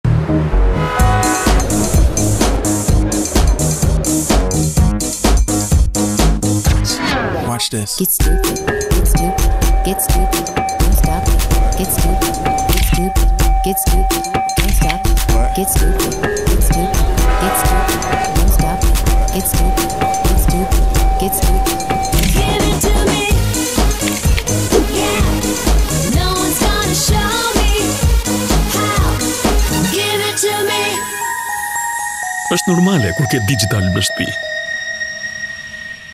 DIGITALB - Eshte normale kur e ke ne shtepi_TVC 4
Konceptimi dhe regjia e serise se ketyre TVC,
me rastin e 4 Vjetorit te Platformes DIGITALB.
Tingelluan vertet te vecanta ne tregun komercial
shqipar.

Creative & Art Director: G. Gjineci